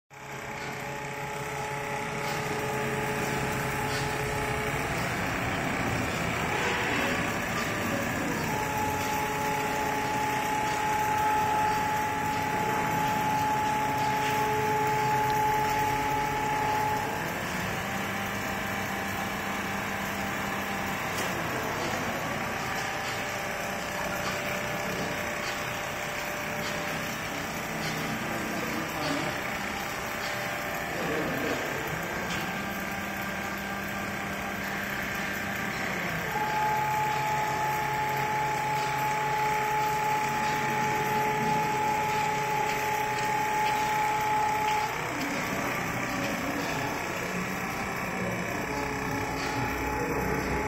Welcome customer visit our factory--Computerized single needle quilting machine
Welcome customer visit our factory, check QY-26 single needle quilting machine, thanks for your trust.